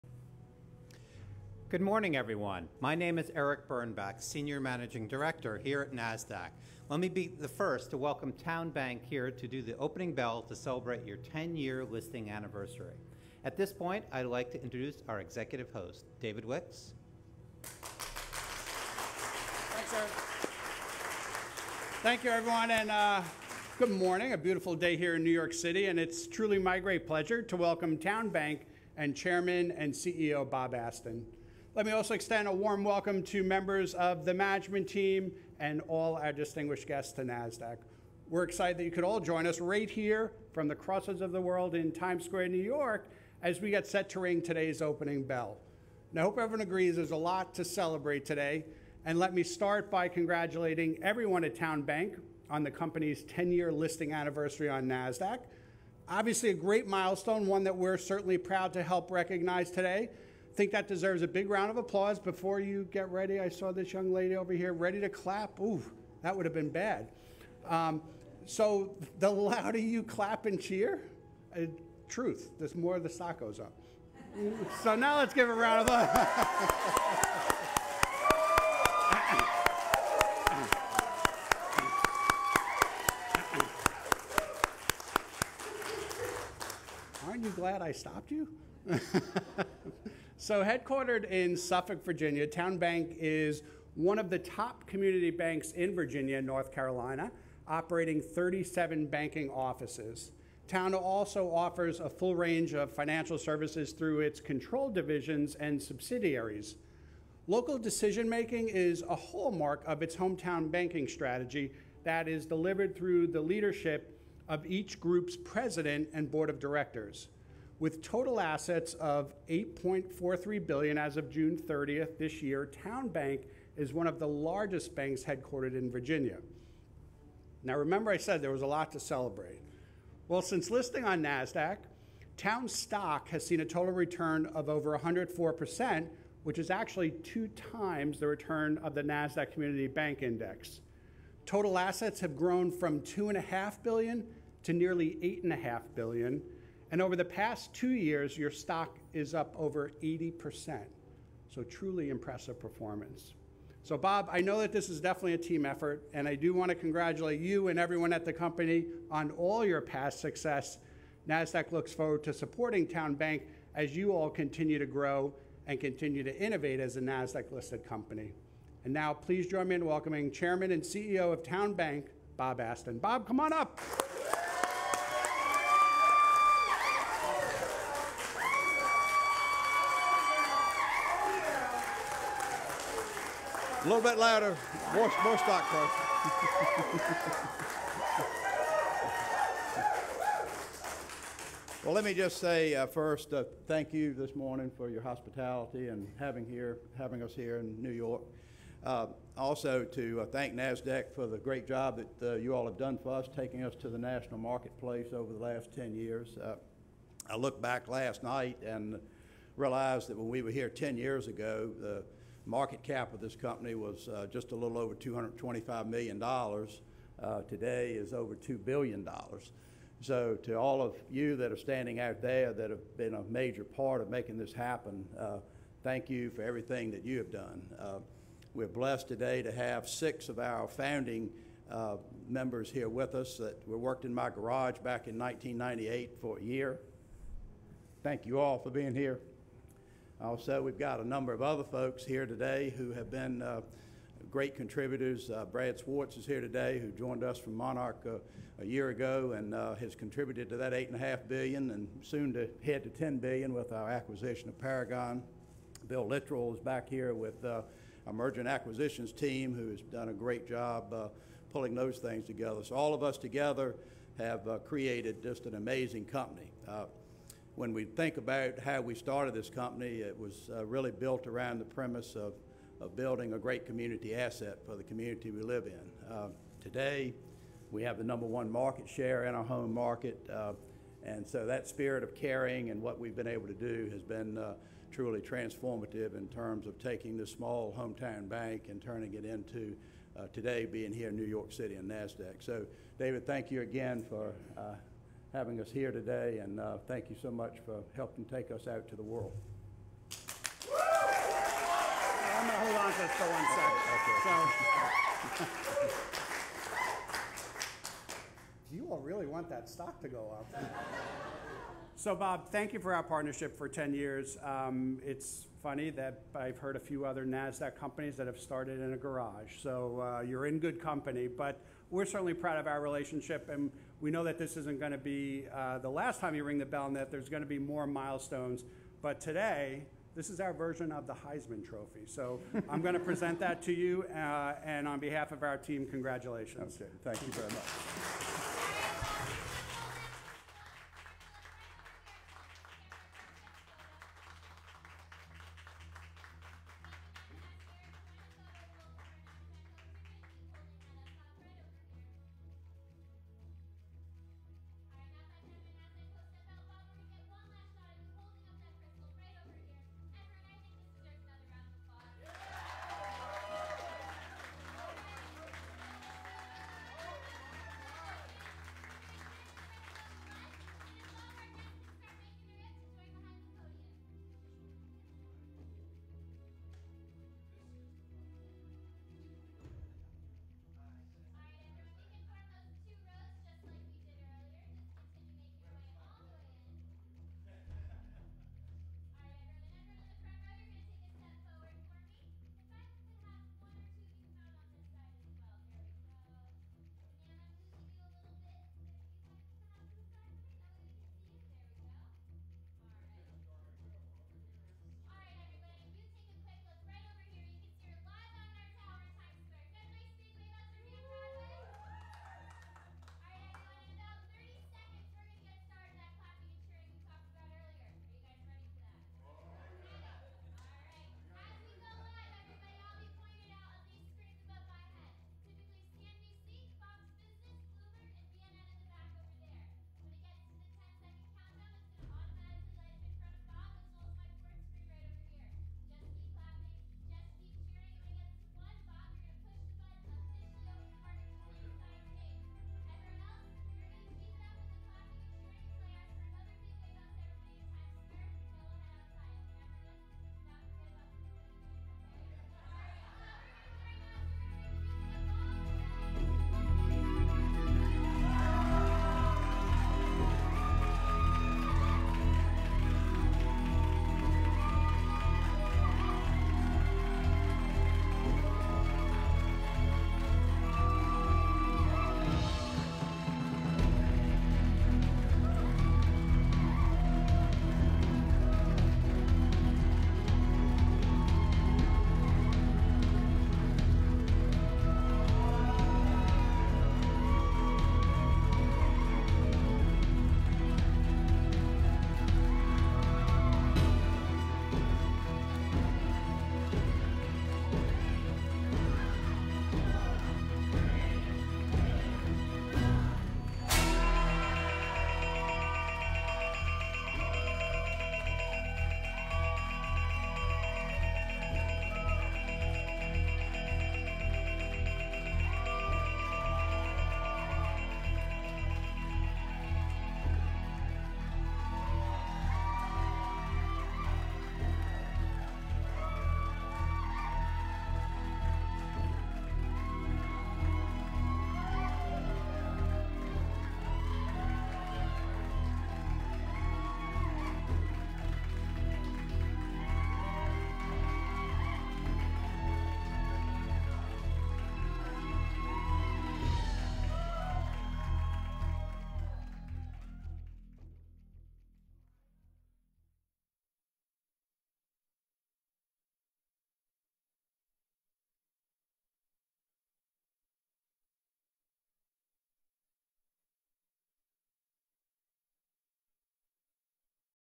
10 Years on Nasdaq! TowneBank Rings Opening Bell
TowneBank CEO Bob Aston marks the 10th anniversary of $TOWN's listing on Nasdaq by ringing the opening bell on October 10, 2017. The stock was listed originally on October 9, 2007.

Member FDIC. Equal Housing Lender.